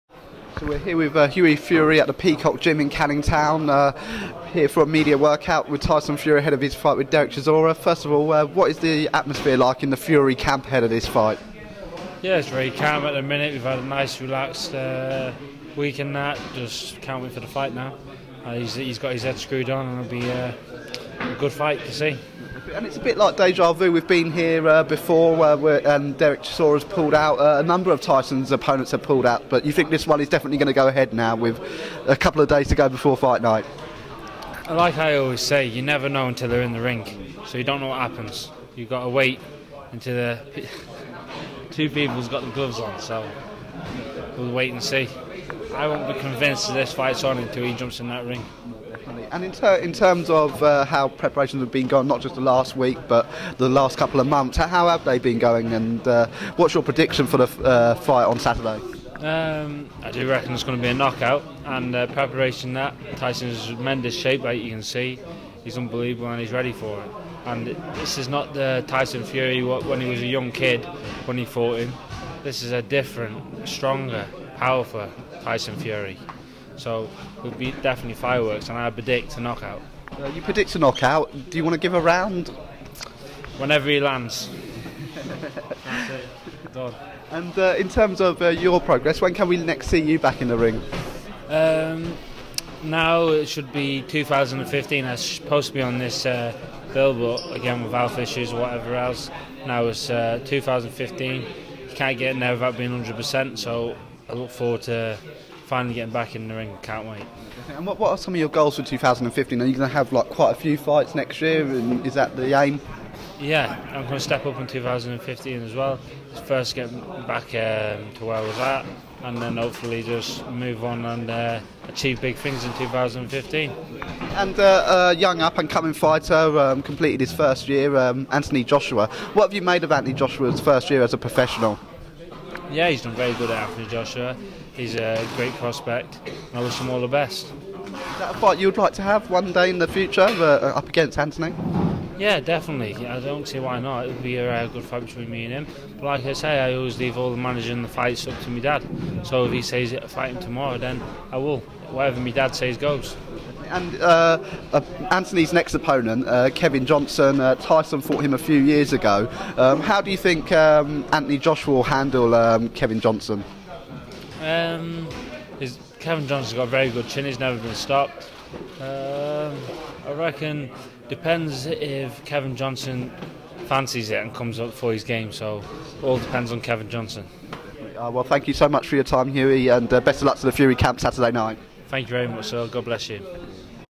Hughie Fury Talks Tyson Fury v Dereck Chisora
Hughie Fury chats to Seconds Out about the Tyson Fury v Dereck Chisora bout this Saturday at the Excel Arena in London

Enter Description Here